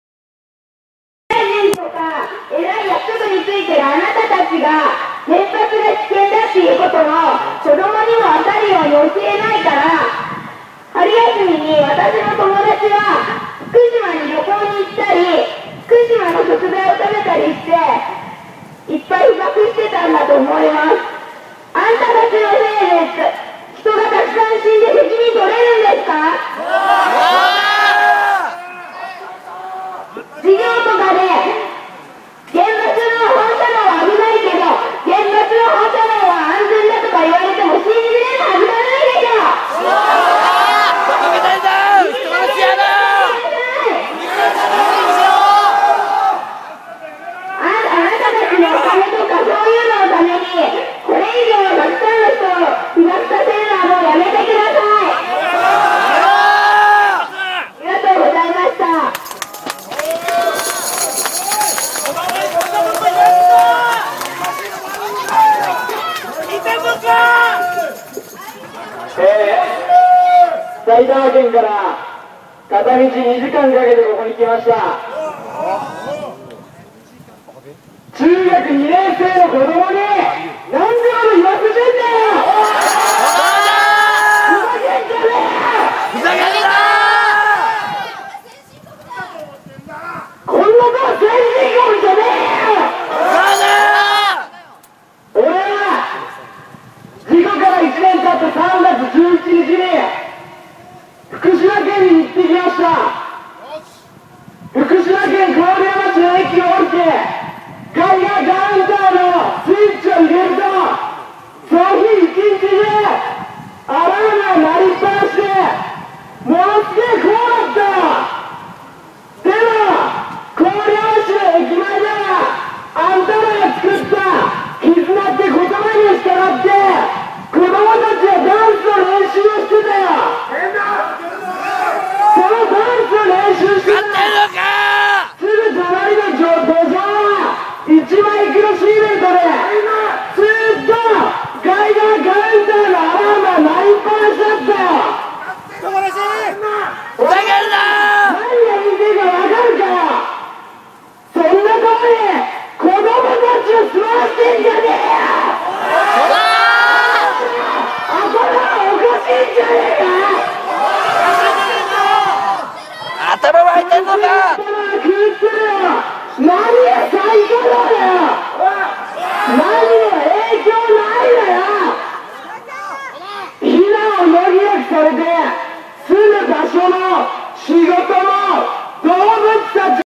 官邸前抗議行動 中学2年の子のスピーチ 4/27
毎週金曜官邸前で抗議活動をしています！

0:00〜0:58　は中学2年生の女の子

偉い役職に就いているあなた達が原発が危険だってことを子供にも分かるように教えないから、春休みに私の友達は福島に旅行に行ったり福島の食材を食べたりして、いっぱい被曝してたんだと思います。
あんたちのせいで人が沢山死んで責任取れるんですか？
授業とかで原爆の放射能は危ないけど、原発の放射能は安全だとか言われても信じれるはずがないでしょ。いい加減にして下さい。あなた達はお金とかそう言うのの為にこれ以上沢山の人を被曝させるのはやめて下さい。

1:08〜3:00　は埼玉県の男性

埼玉県から片道2時間かけてきました。
中学2年生の子供になんてこと言わせてんだよ、ふざけんじゃねえよ、こんなとこ先進国じゃねえよ。
俺は事故から１年経った3月17日に福島県に行って来ました。
福島県郡山市の駅を出てガイガーカウンターのスイッチを入れると、その日一日中アラームが鳴りっぱなしでものすごく怖かったよ。
でも郡山市の駅前では、あんたらの作った絆って言葉に従って、子供達がダンスを練習してたよ。そのダンスを練習してるののすぐ隣の土壌は1マイクロシーベルトで、ずっとガイガーカウンターのアラームが鳴りっぱなしだったよ。俺が何がいいてえか分かるかよ。そんなところに子供達を住まわしてんじゃねえよ。頭がおかしいんじゃねえか。